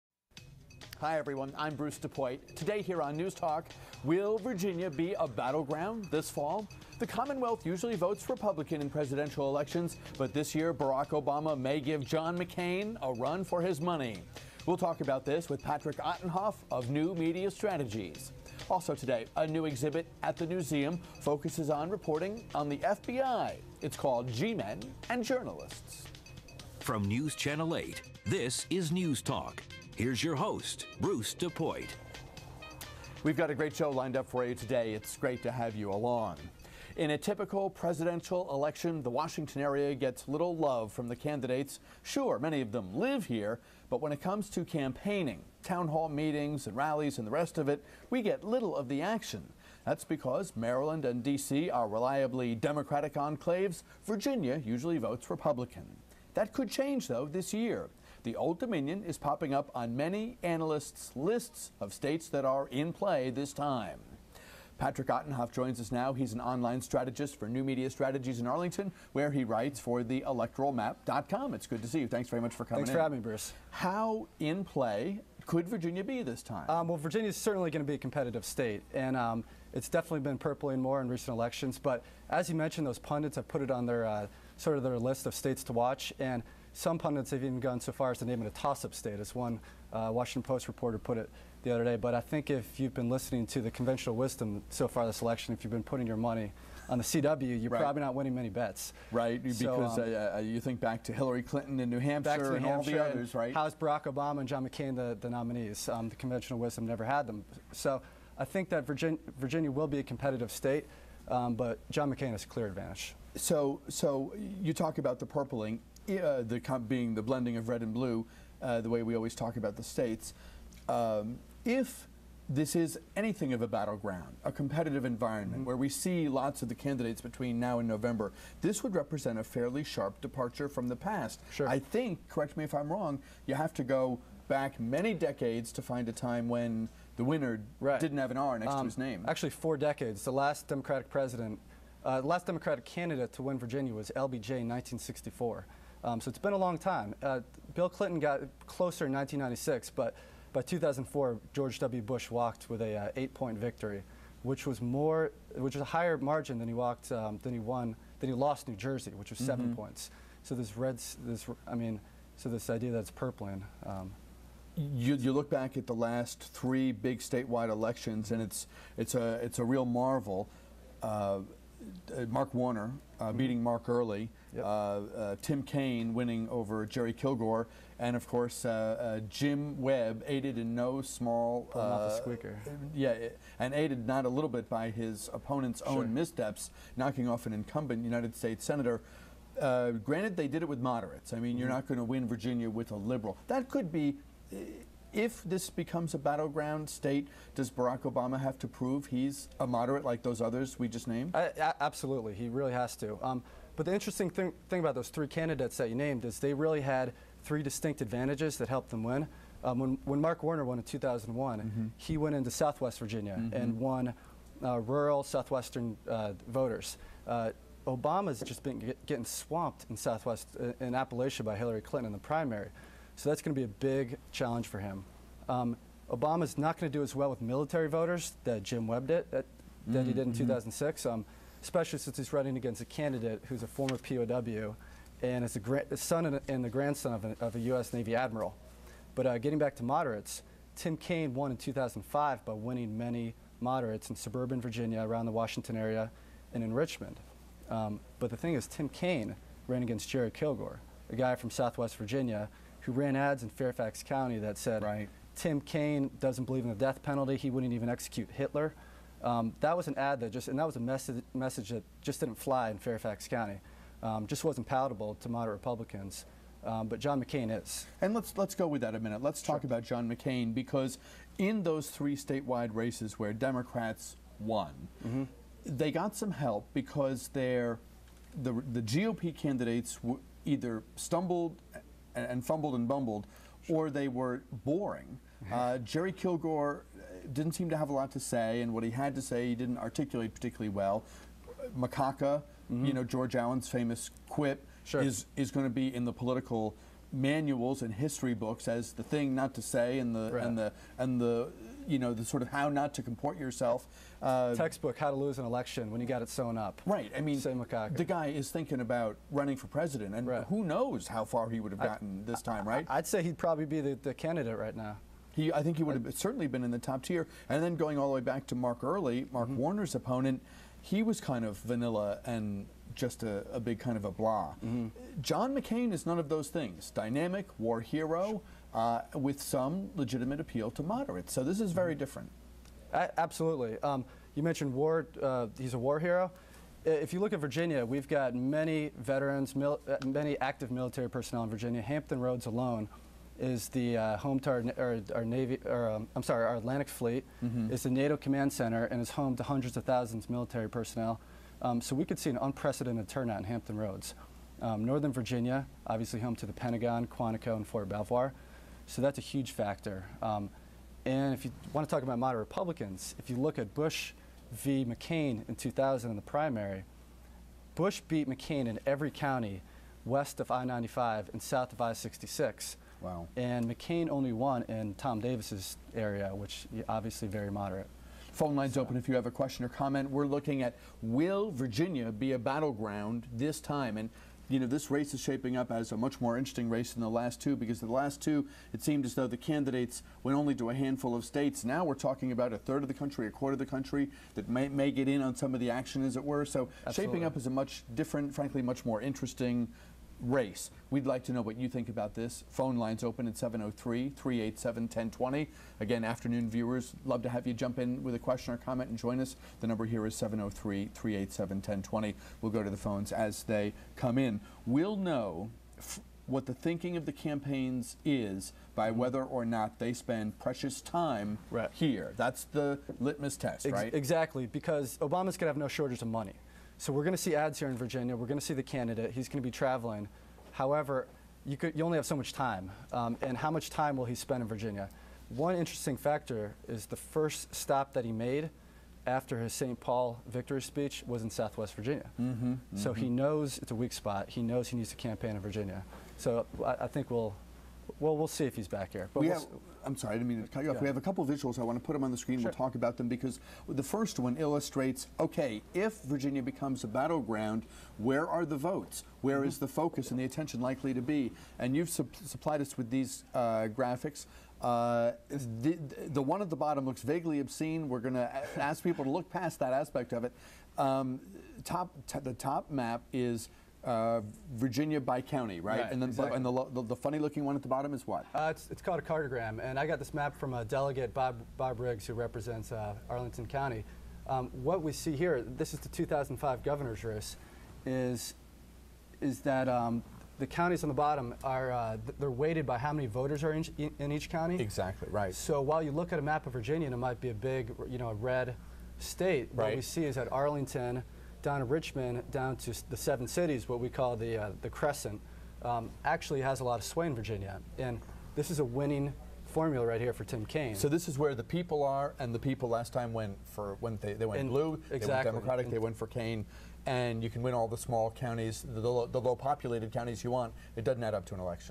Ottenhoff on News Channel 8 Part 1 of 2
Patrick Ottenhoff talks about Virginia in 2008.